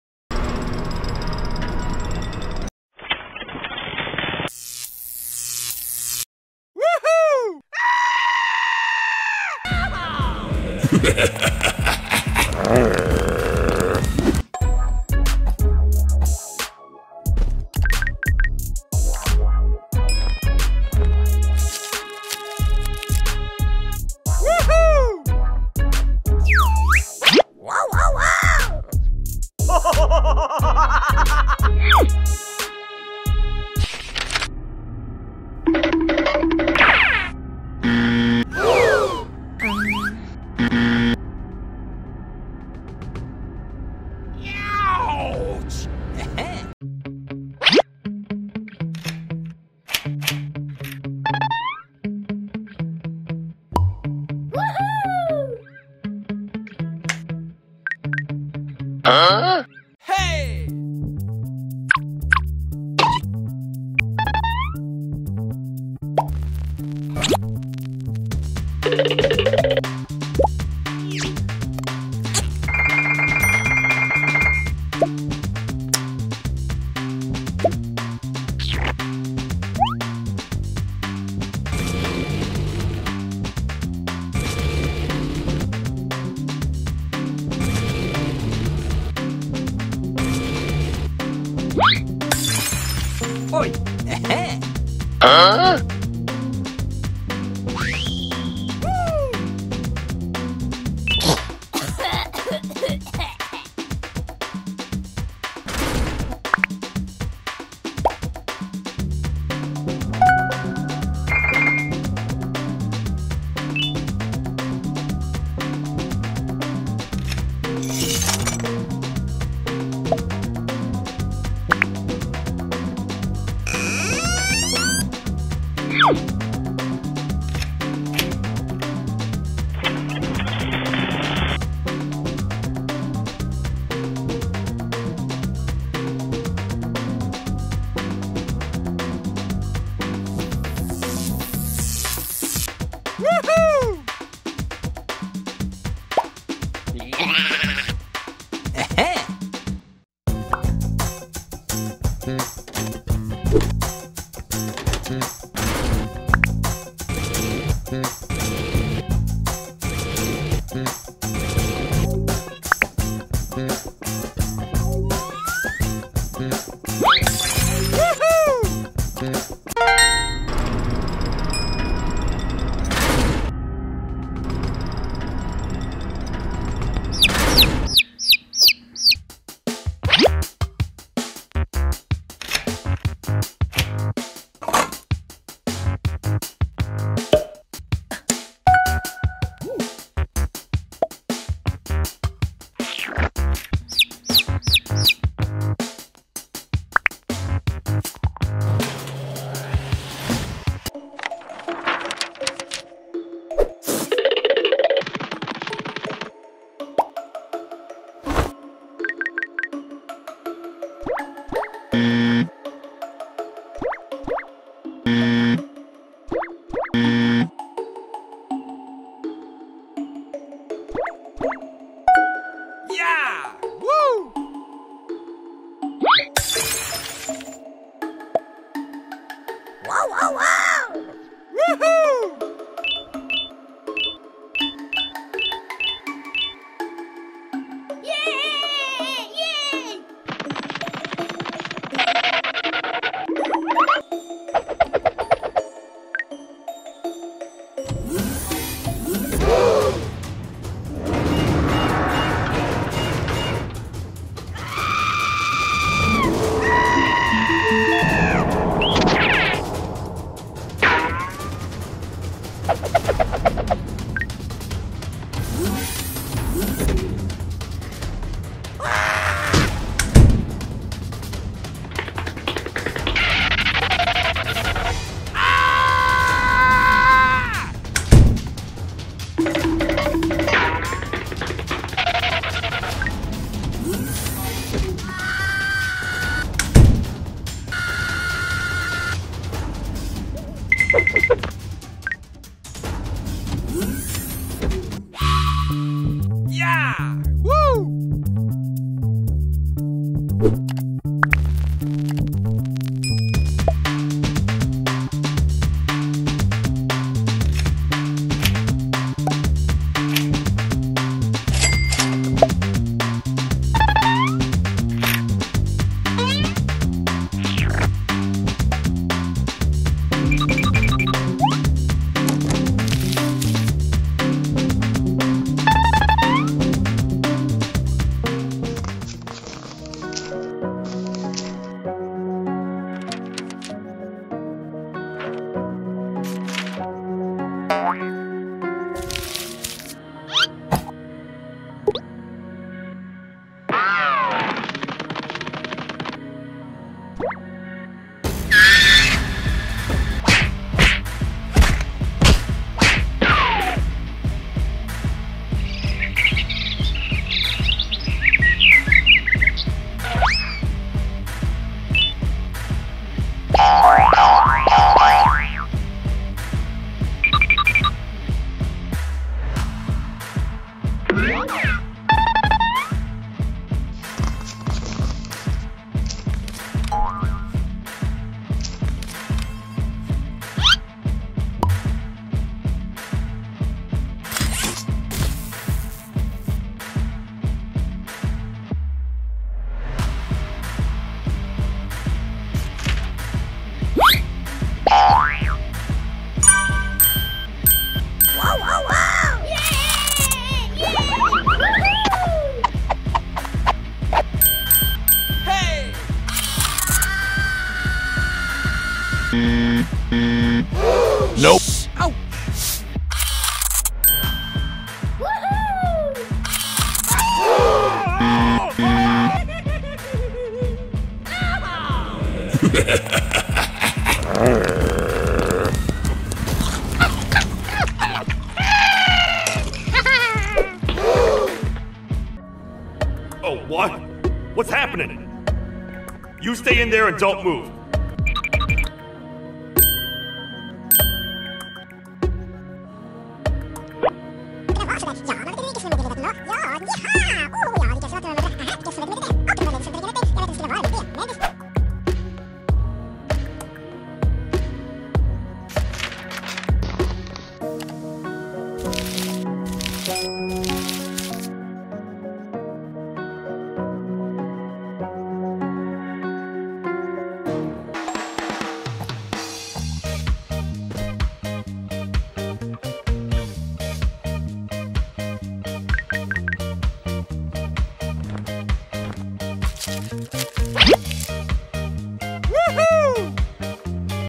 NEW GARTEN OF BANBAN 5 BitterGiggle VS Sheriff Toadster Gaming Book/ Nabnab Tamataki & Chamataki
NEW GARTEN OF BANBAN 5 BitterGiggle Pregnant with Sheriff Toadster or Queen Bouncelia Gaming Book

Printed versions of this book are now available for purchase.
The price is $6.
Write to the mail
Thank you for your views and thanks